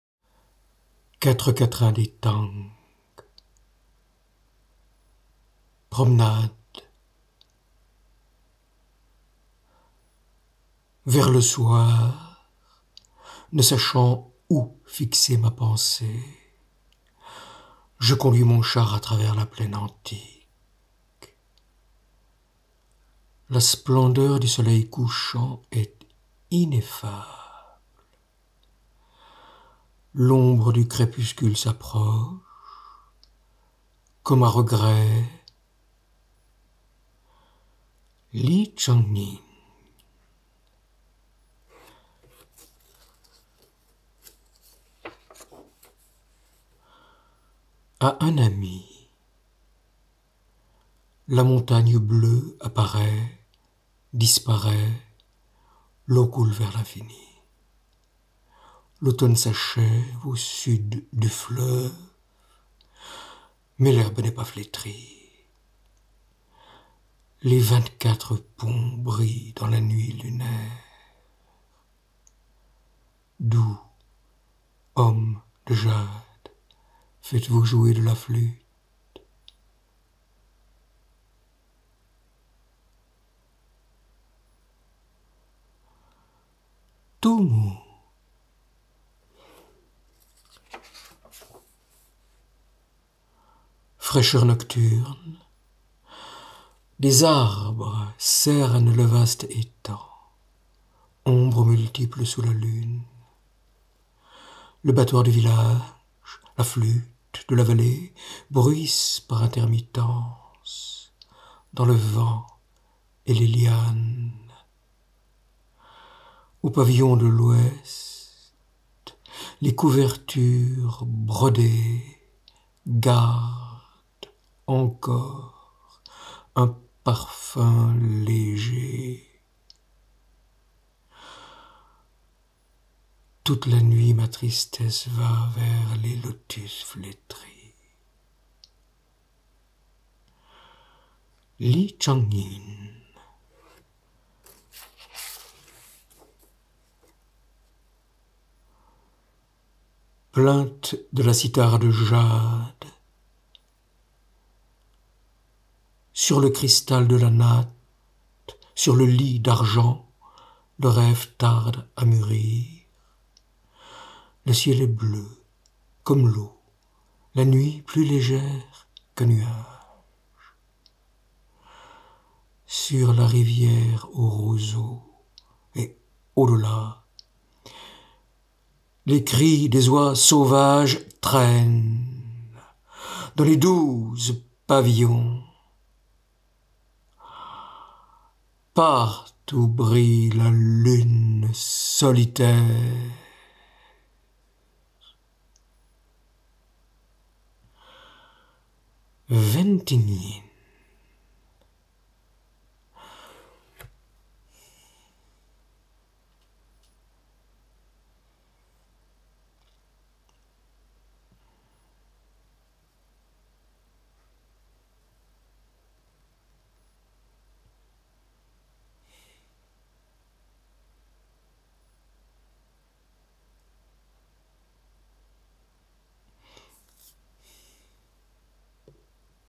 Quatrains chinois. Quatre en lecture. De «Cent quatrains des T’ang» / 6
De « Cent quatrains des T’angs », quatre quatrains chinois lus par Jean-Yves Dubath --- Car ce livre, « Cent quatrains des T’angs », traduits du chinois par Lo Ta-Kang (édité à La Baconnière – Neuchâtel, en 1947), ce livre avait été offert ma mère dans sa jeunesse ; il se trouvait dans la bibliothèque familiale ; je l’ouvrais, ce n’était qu’une étrangeté, froide, peu peuplée ; puis, avec les années, passant, les phrases ont commencé à bouger ; un vol de canard est devenu comme une manière d’apothéose, et ainsi de suite – et tant qu’à la fin, aujourd’hui, dans ces quatrains, tout vibre, tout s’anime et à chaque lecture renouvelée. Nulle idée de ce qu’il en sera demain ; c’est toujours ainsi.
Because this book, "Cent quatrains des T’angs", translated from Chinese by Lo Ta-Kang (published at La Baconnière - Neuchâtel, in 1947), this book had been offered to my mother in her youth; he was in the family library; I opened it, it was only a strangeness, cold, sparsely populated; then, as the years went by, the sentences started to move; a flight of duck became like a kind of apotheosis, and so on - and as long as at the end, today, in these quatrains, everything vibrates, everything comes alive and with each renewed reading. No idea what will happen tomorrow; it’s always like this.